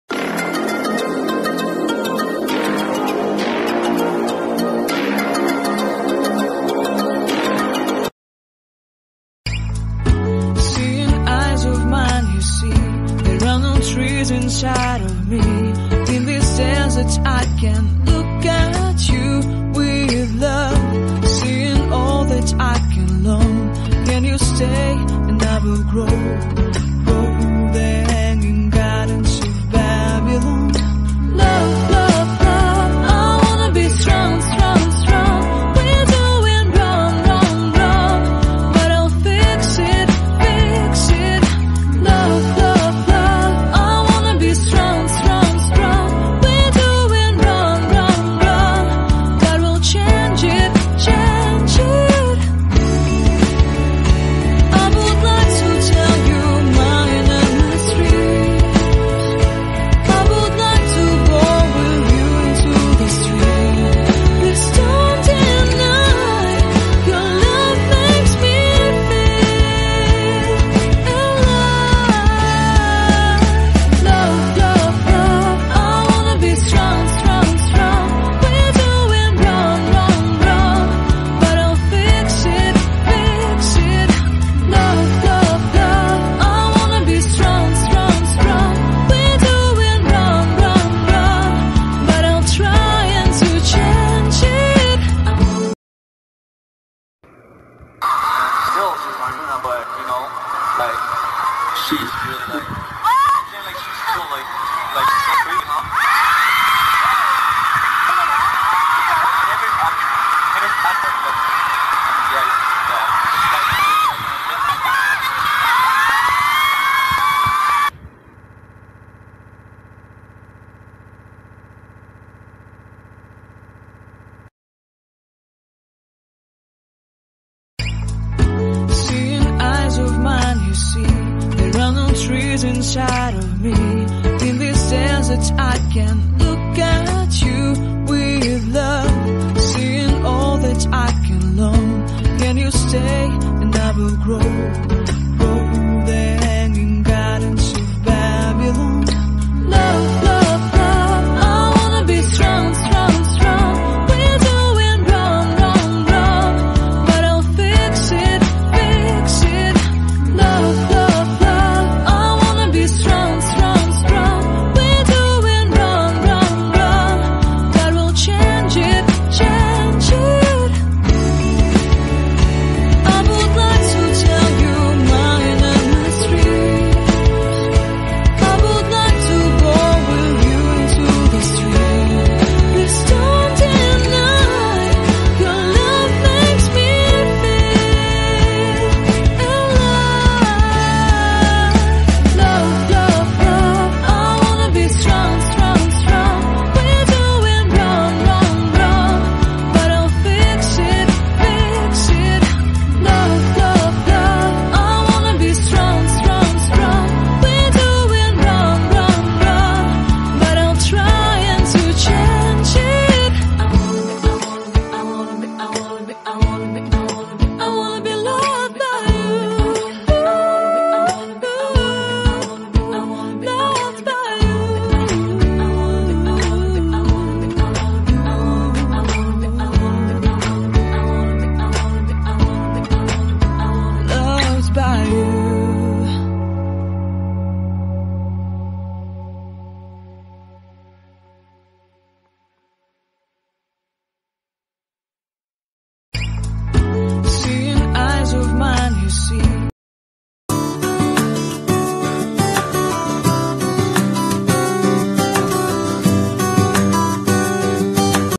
The DARAGON LOVE STORY!!!!STILL IN 2020?
Here to all their fans that they really dating up until now!